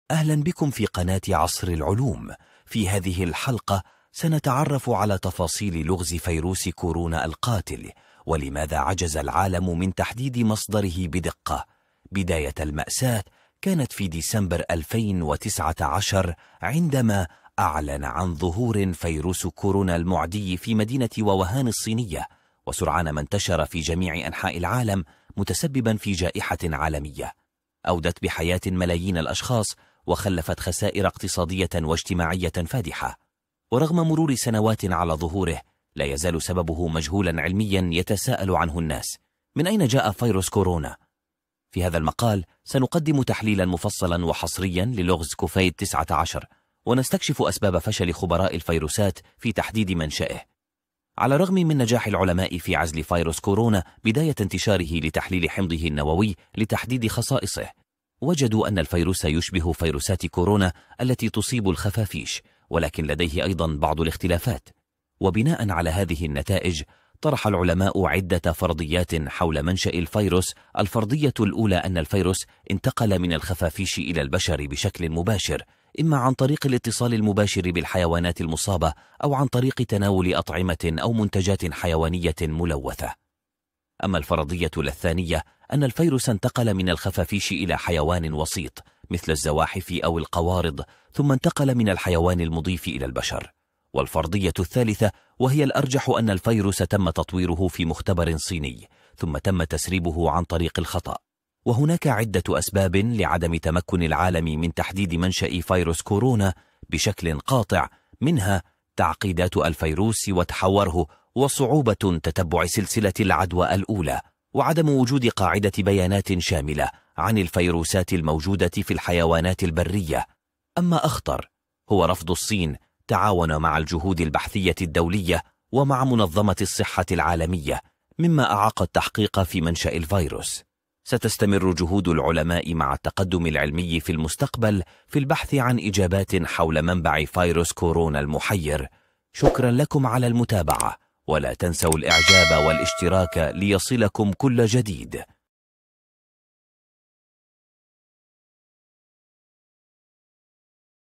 فيروس كورونا: اكتشف معنا أسباب استمرار الغموض في أصل وباء في فيديو حصري
هل تساءلت يومًا عن سبب عدم تحديد أصل فيروس كورونا؟ في هذا الفيديو، سأقدم لك إجابات شاملة ومثيرة للاهتمام حول هذا الموضوع المثير للجدل. ستكتشف الحقائق والأدلة التي تشير إلى أسباب عدم تحديد مصدر هذا الفيروس المستجد. انضم إلينا واكتشف الحقيقة وراء هذه الظاهرة العالمية التي أثارت تساؤلات كثيرة. استعد للانغماس في رحلة معرفية مثيرة والعديد من التساؤلات المحيرة حول هذا الموضوع الهام. فلا تفوت فرصة التعرف على التطورات الأخيرة والأبحاث المستقبلية في هذا المجال. انضم إلى مجتمعنا وانطلق في رحلة استكشافية معنا!
في هذا الفيديو، سنكشف عن الأسباب العلمية التي أعاقت تحديد أصل فيروس كورونا، وهو لغز محير يحير العلماء والخبراء منذ ظهوره في عام 2019
ما هو فيروس كورونا باختصار؟ما هي طرق الوقاية من فيروس كورونا؟هل فيروس كورونا مرض؟ما معنى فيروس؟